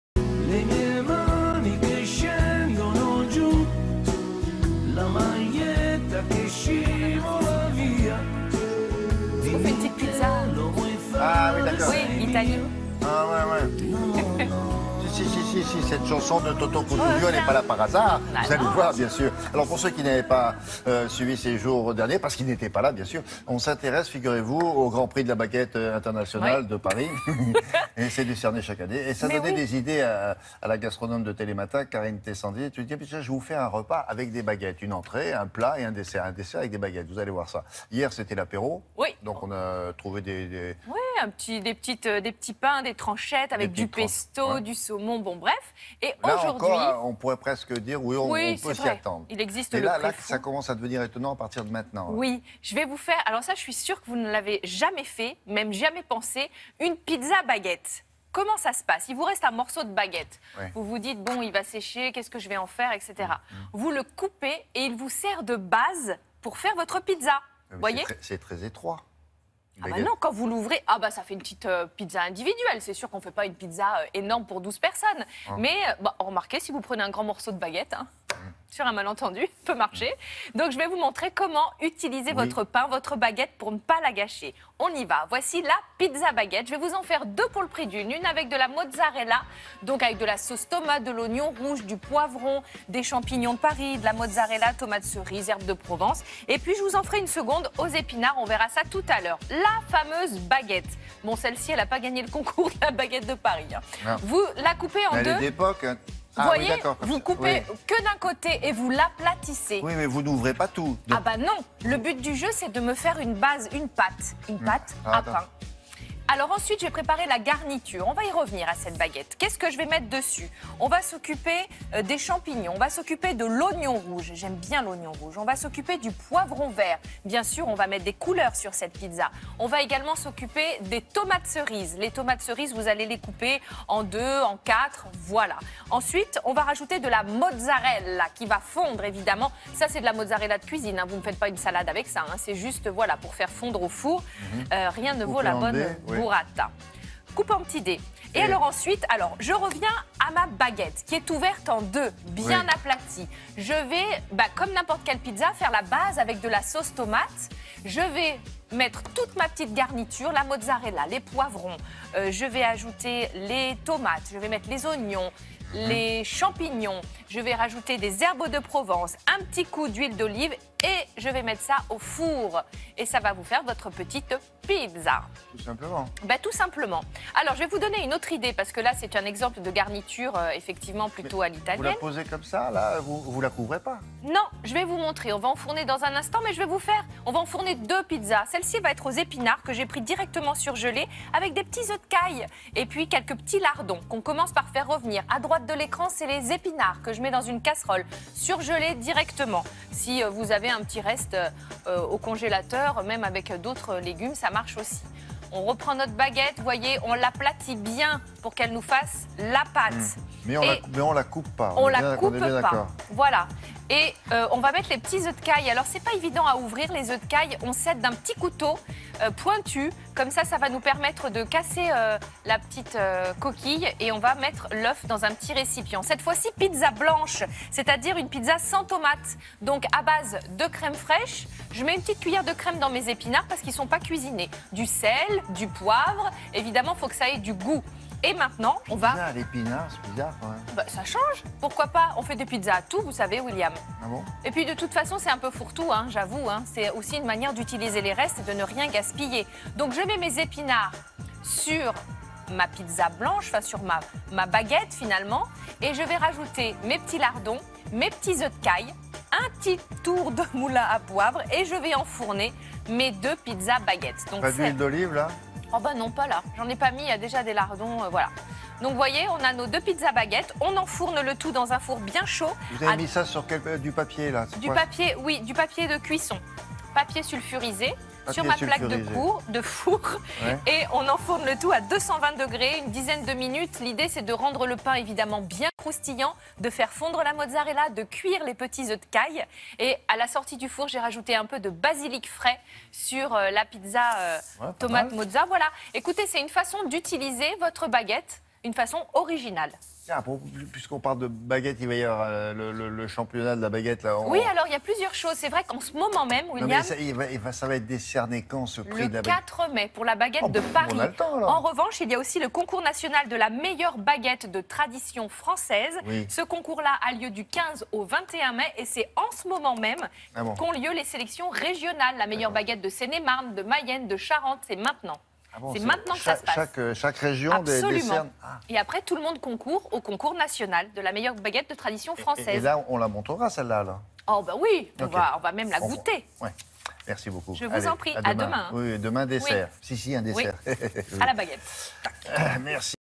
Gourmand - Pizza baguette tomate mozza
Notre chroniqueuse Carine Teyssandier notre prépare une délicieuse recette.